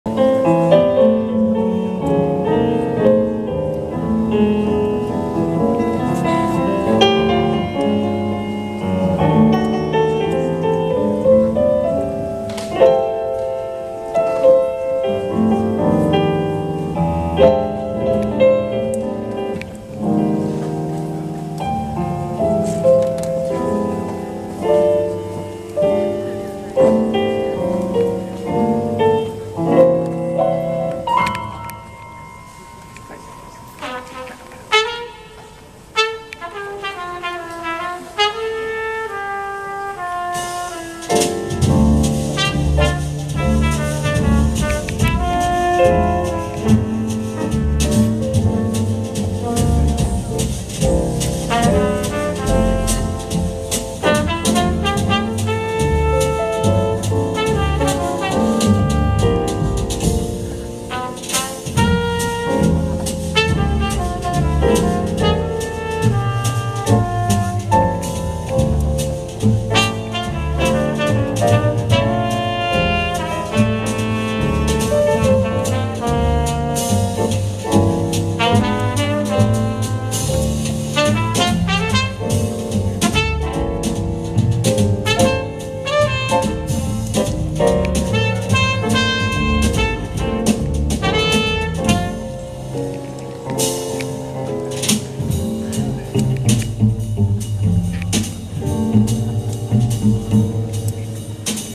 Joe Magnarelli Quartet
Fragmento de la actuación del cuarteto de Joe Magnarelli en la UNIA. Septiembre de 2010, Monasterio de la Cartuja, Sevilla